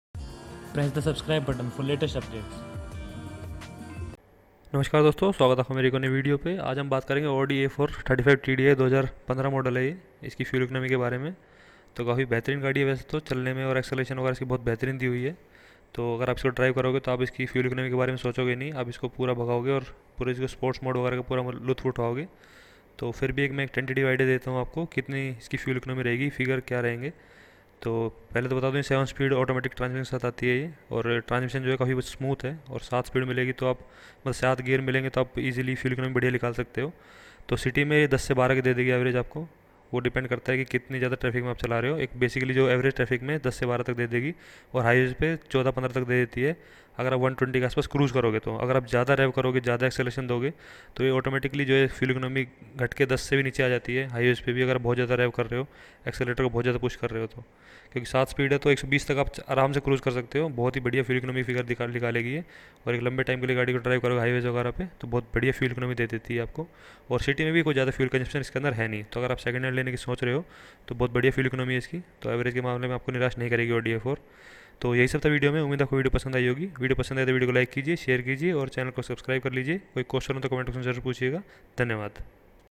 Audi A4 35 TDi 2015 Model Fuel Economy | Reallife Mileage Of Audi A4
in this video i have discussed reallife fuel economy mileage of audi a4 35tdi 2015 model . audi a4 35tdi comes with 7 speed automatic transmission and delivers good fuel economy . the fuel economy of audi a4 35tdi 2015 model in city is around 10-12 kmpl and on highway 14-15 kmpl . basically the fuel economy of any vehicle depends on the various factor . so it may vary from person to person and terrain to terrain . so the fuel economy or average that i have discussed about audi a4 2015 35 tdi is just to give you all an idea about the fuel economy it generally delivers .